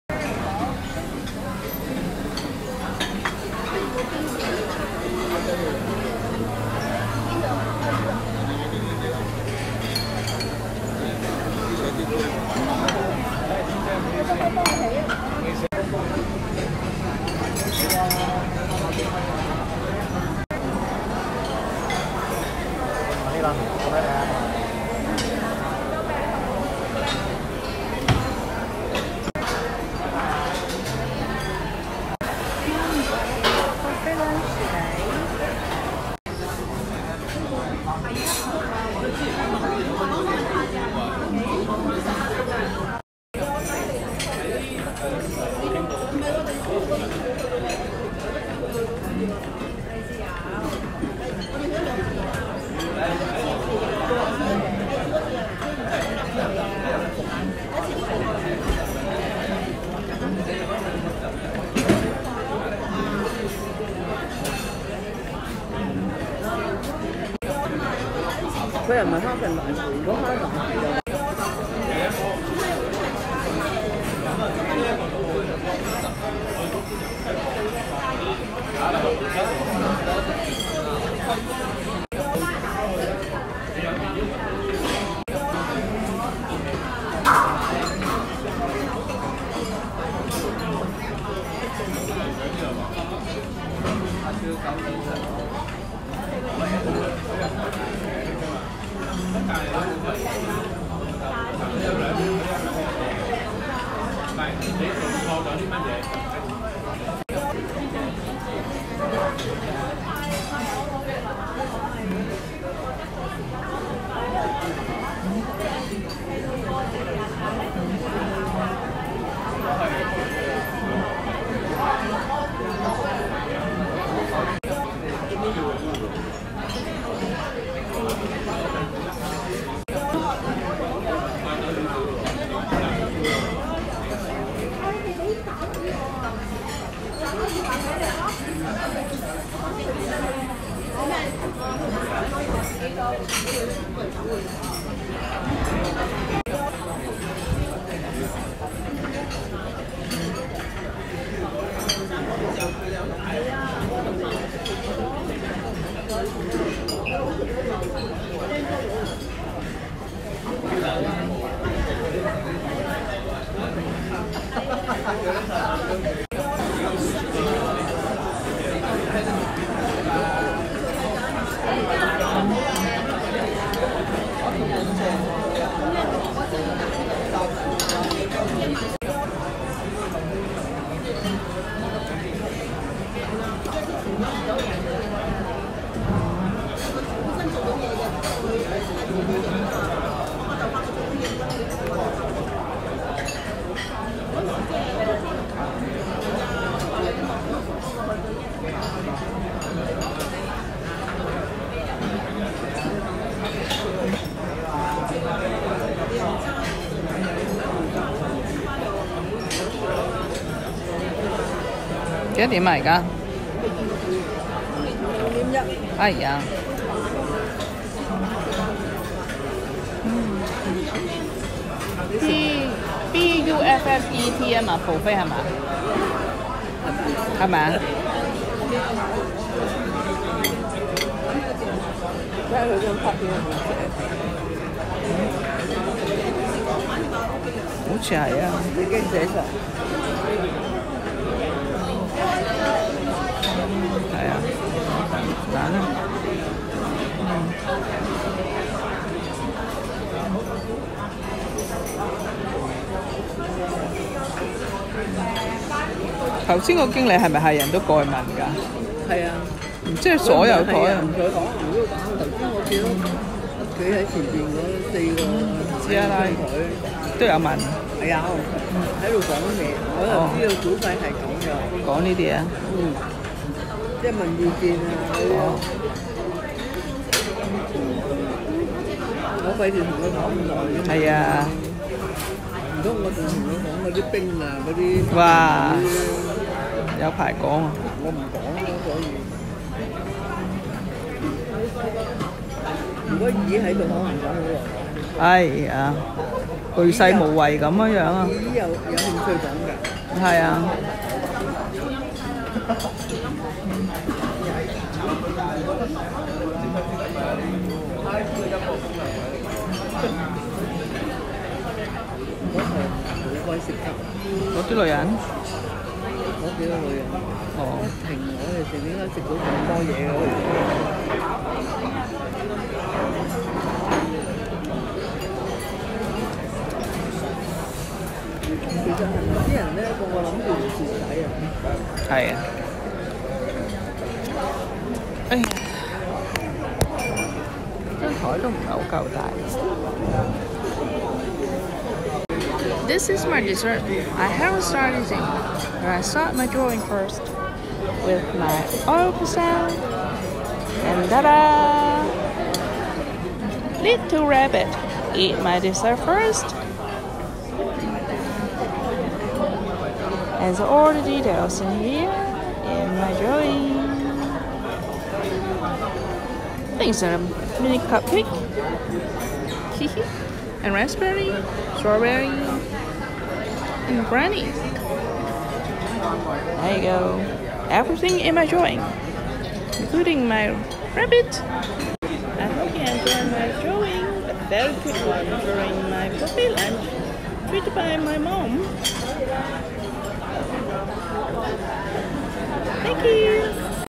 Drawing at buffet 🍰🍧🍨
Live drawing at buffet 😸 mom treated me buffet which I haven’t tasted for quite a while😆 bringing  my art material n bunny doll to do a quickie drawing there 😄 enjoy everyone for the lovely dessert 🍮